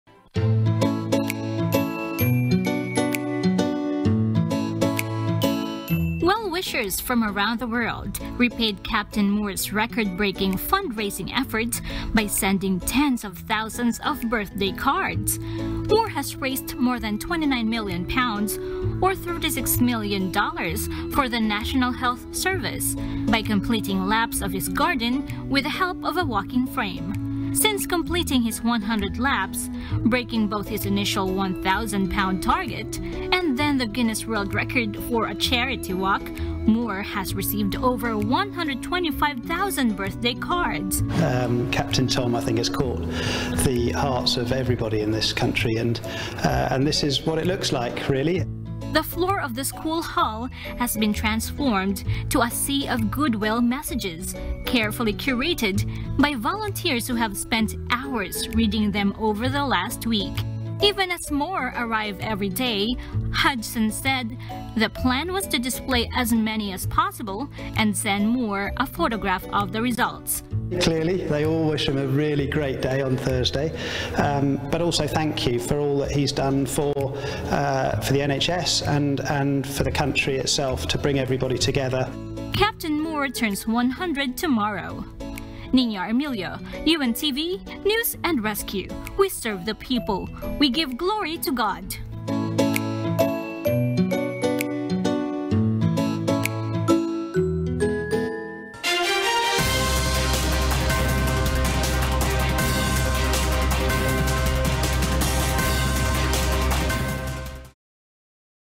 Birthday tributes flood in for fund-raising British war veteran 'Captain Tom'
WATCH: British World War Two veteran and Guinness Book of World Records title holder Captain Tom Moore  is in for a very special 100th birthday on Thursday.